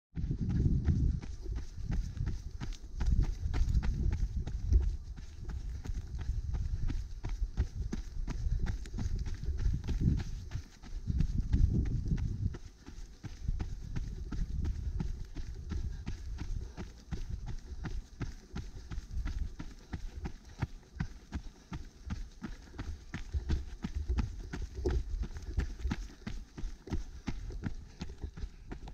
Tankwa Trail Run - Day 3 - Part of 15km trail
This is a short taster for day 3 of the Tankwa Trail Run, a free 3-day traill run in the Tankwa Karoo.